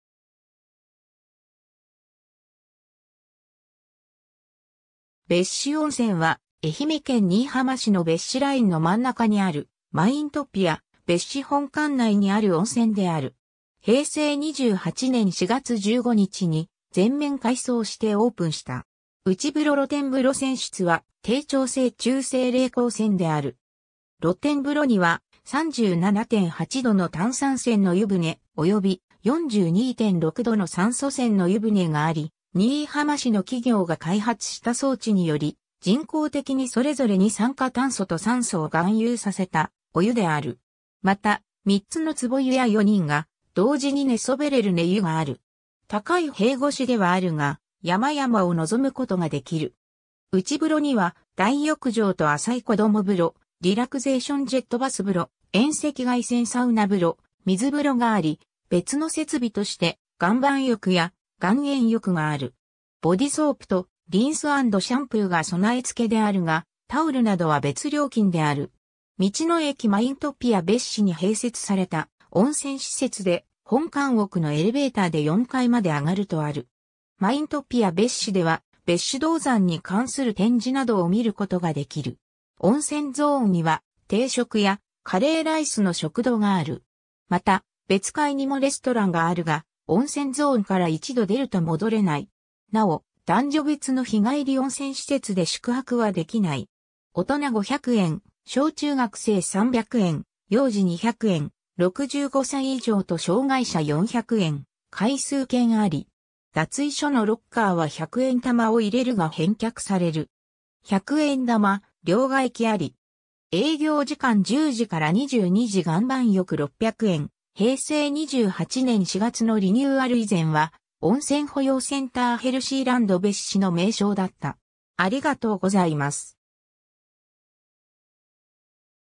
別子温泉
別子温泉（べっしおんせん）は、愛媛県新居浜市の別子ラインの真ん中にあるマイントピア別子本館内にある温泉である。
平成28年4月15日に、全面改装してオープンした。
 内風呂 露天風呂 泉質は、低張性中性冷鉱泉（平成26年5月26日分析）である。
露天風呂には、37.8度の炭酸泉の湯船および、42.6度の酸素泉の湯船があり、新居浜市の企業が開発した装置により人工的にそれぞれ二酸化炭素と酸素を含有させたお湯である。
また、3つの壺湯や４人が同時に寝そべれる寝湯がある。
高い塀越しではあるが、山々を臨むことができる。
内風呂には、大浴場と浅い子供風呂、リラクゼーションジェットバス風呂、遠赤外線サウナ風呂、水風呂があり、別の設備として岩盤浴や岩塩浴がある。
ボデイソープとリンス＆シャンプーが備え付けであるが、タオルなどは別料金である。
 道の駅マイントピア別子に併設された温泉施設で、本館奥のエレベーターで４階まで上がるとある。
マイントピア別子では、別子銅山に関する展示などを見ることができる。
温泉ゾーンには定食やカレーライスの食堂がある。
また、別階にもレストランがあるが、温泉ゾーンから一度出ると戻れない。
なお、男女別の日帰り温泉施設で宿泊はできない。
 大人500円（税込）、小中学生300円、幼児200円、65歳以上と障害者400円、回数券あり。
脱衣所のロッカーは100円玉を入れるが返却される。
100円玉両替器あり。
 営業時間 10時から22時（受付終了21時） 岩盤浴600円（50分まで） 平成28年4月のリニューアル以前は、「温泉保養センターヘルシーランド別子」の名称だった。